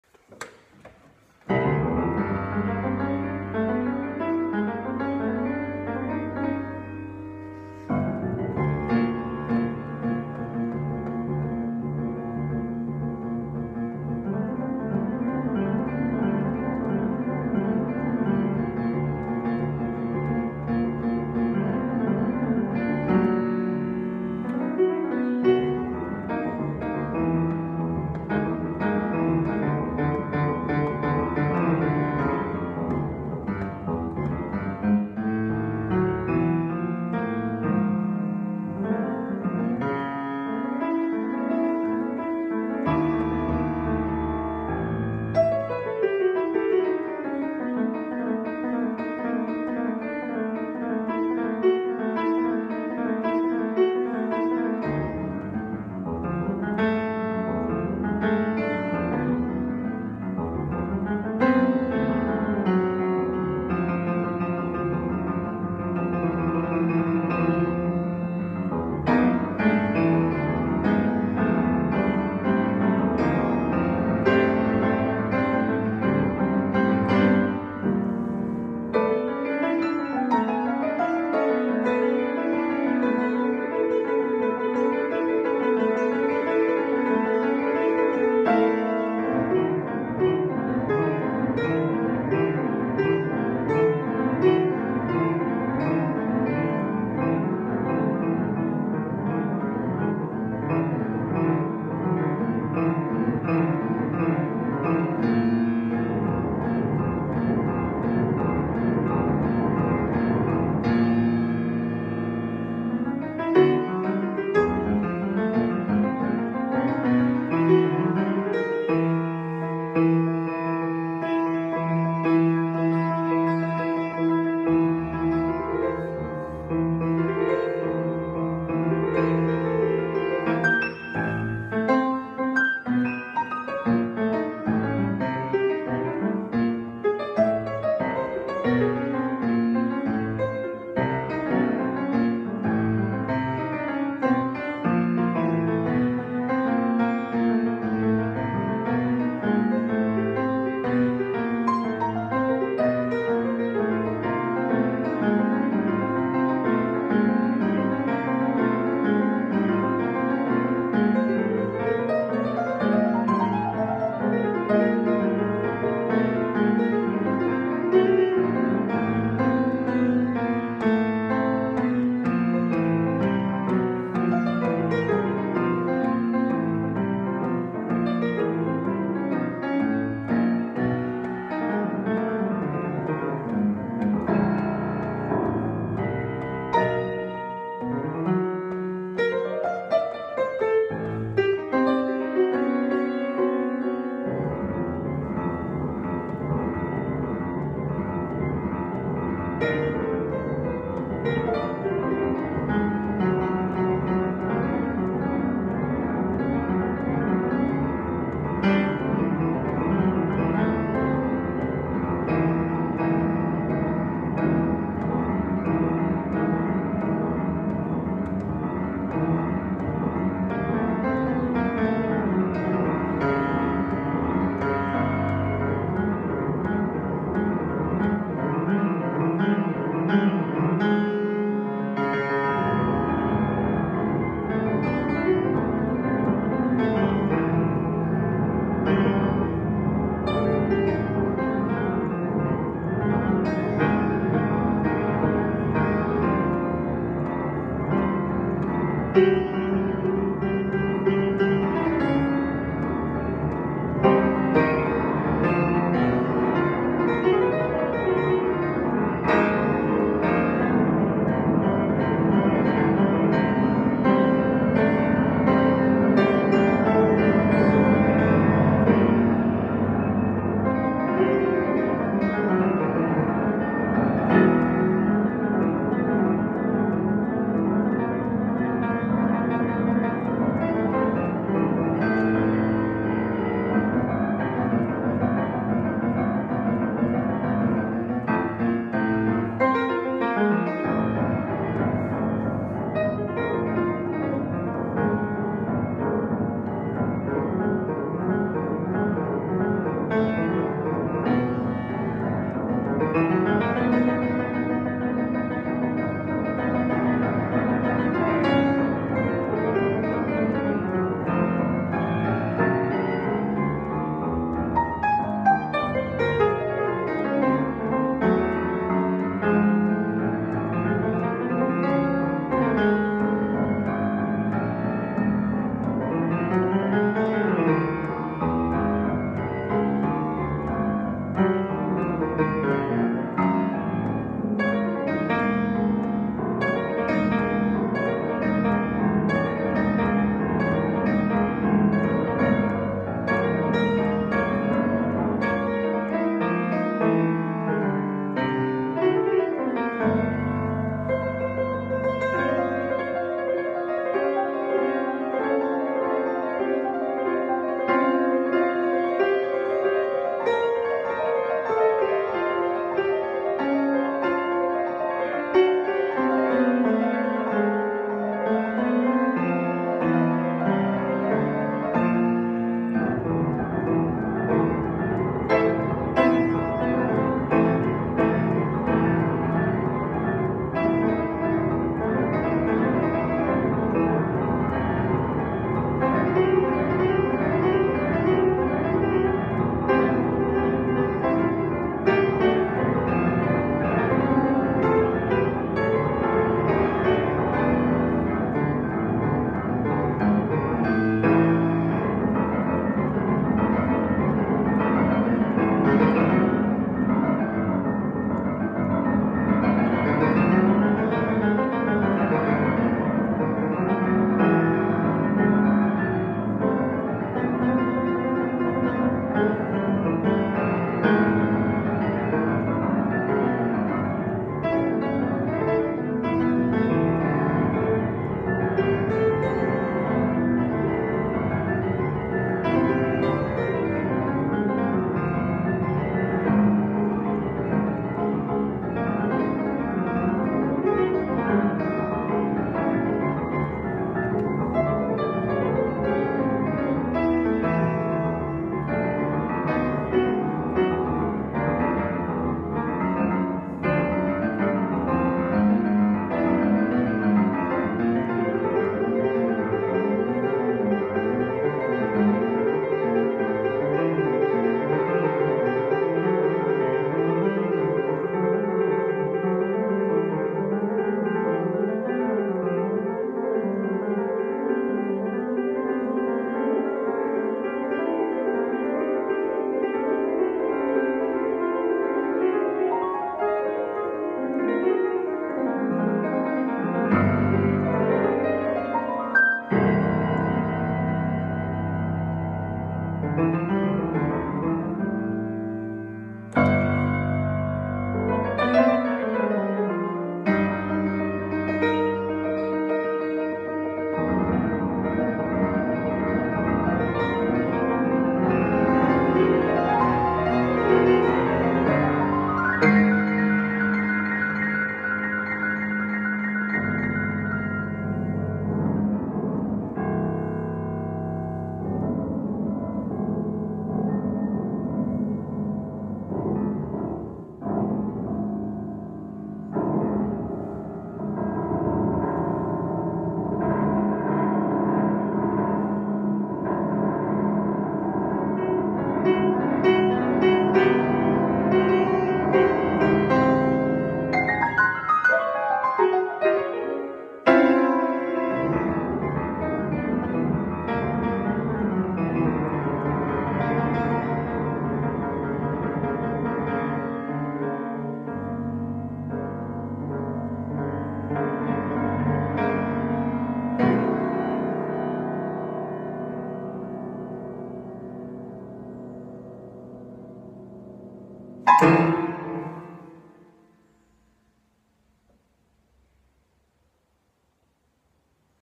An inexhaustible exercise, only limited by imagination.                         Left hand: play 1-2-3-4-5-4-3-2 ... Right hand: play anything  you want.